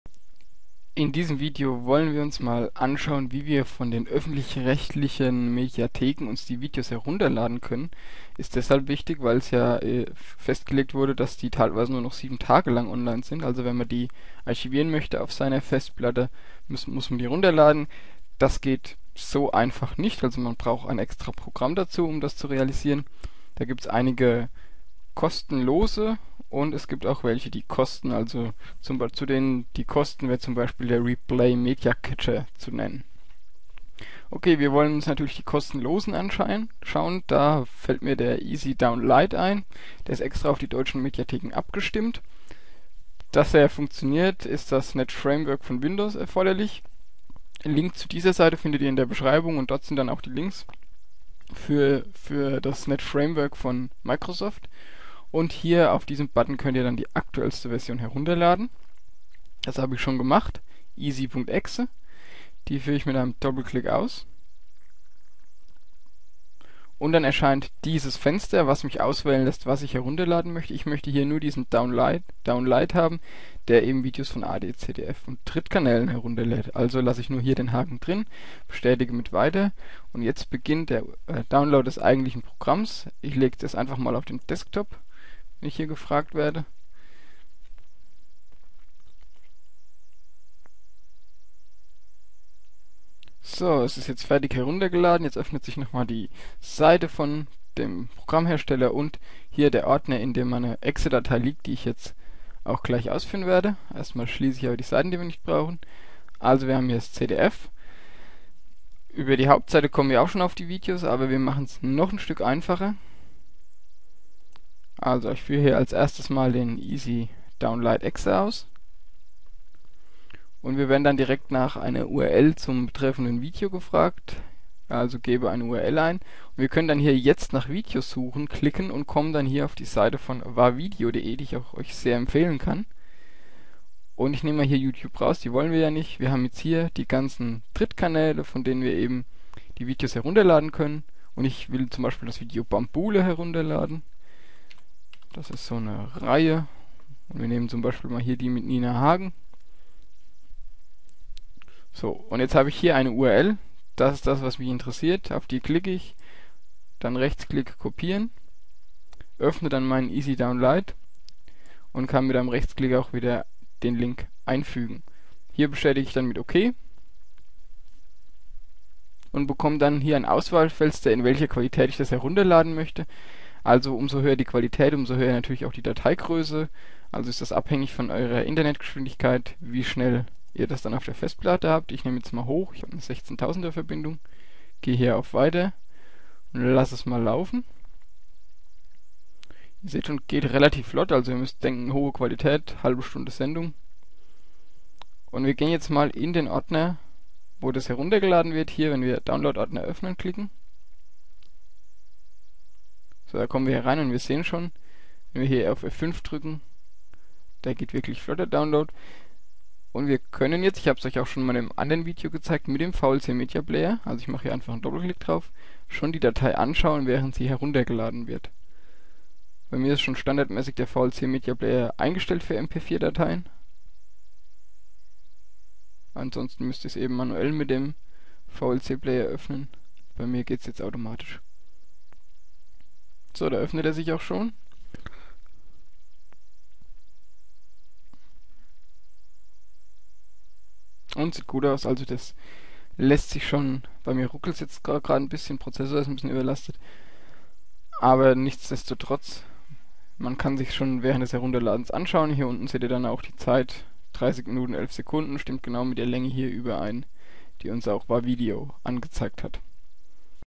[veraltet] Videos von der Mediathek herunterladen / downloaden mit Freeware
Schritt für Schritt Anleitung zum Download von Videos aus den Mediatheken die das RTMP Format nutzen.